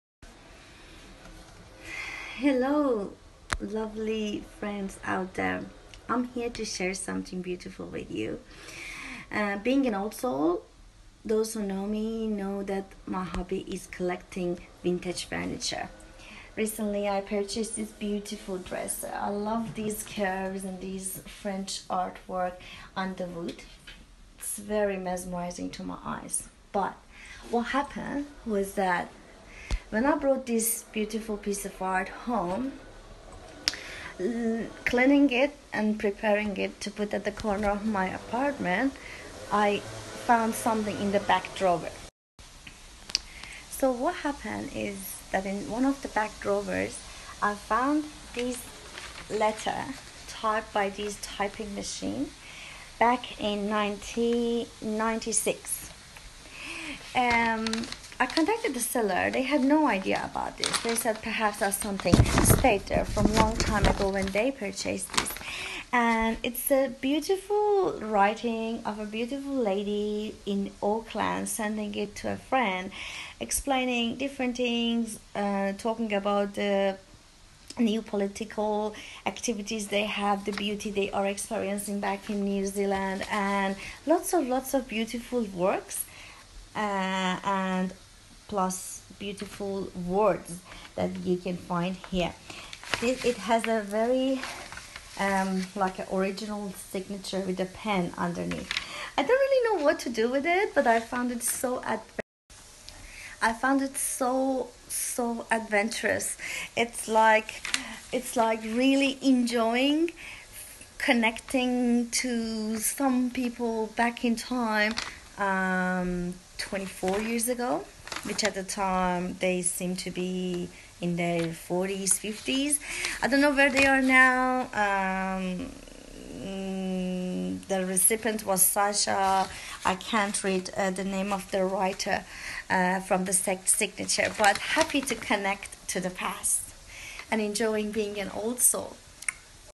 Blast from the Past
Blast from the past- An old soul connects to the past living people through her love to vintage furniture